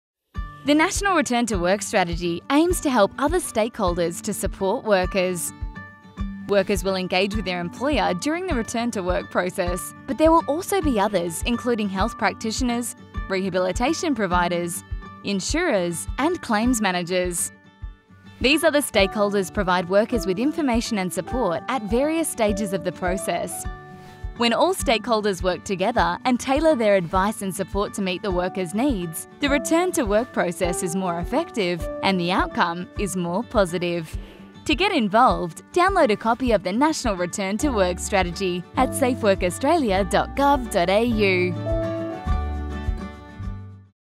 Supporting other stakeholders
Find out how the National Return to Work Strategy is helping other stakeholders support workers in their recovery and return to work.